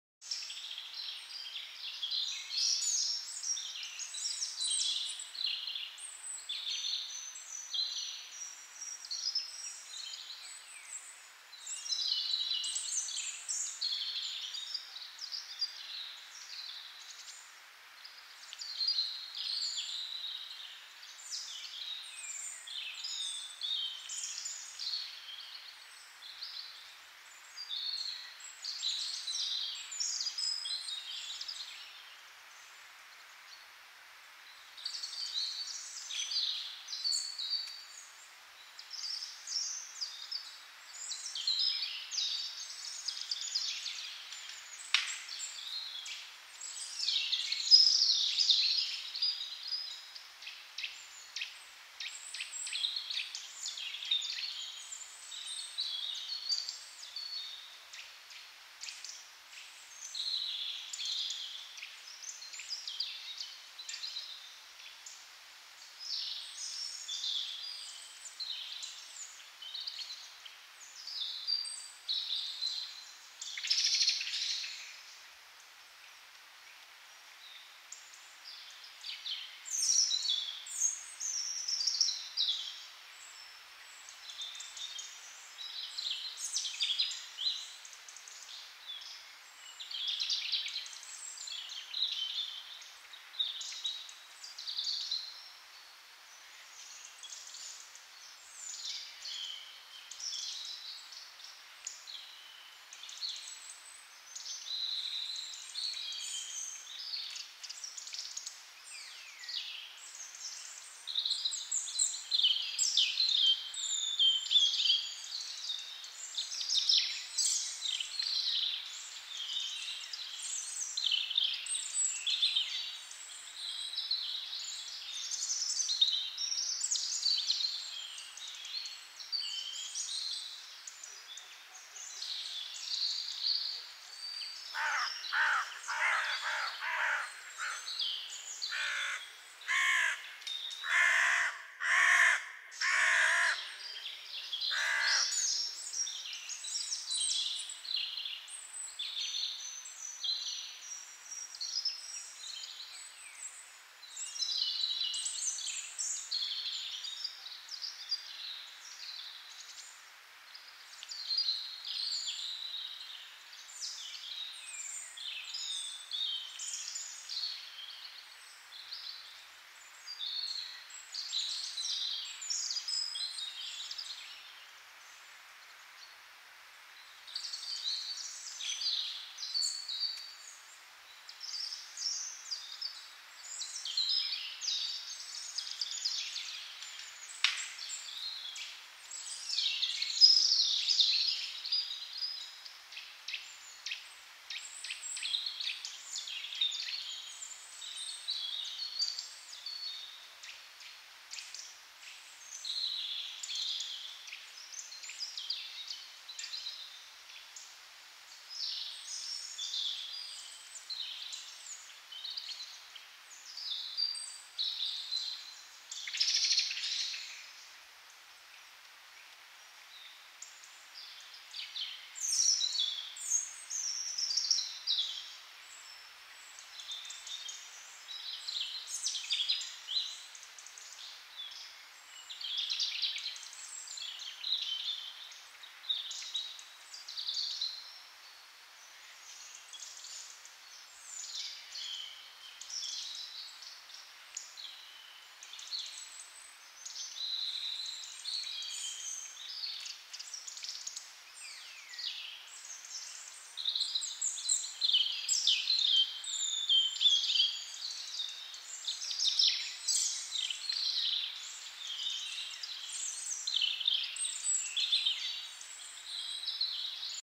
The squirrel Alice eats walnut 🌰🐿️🧡😍🌰
Squirrel Alice came to the feeding station very early. She found half a walnut in the snow, and after eating it, she found a whole walnut, which she took home.Jan-2-2024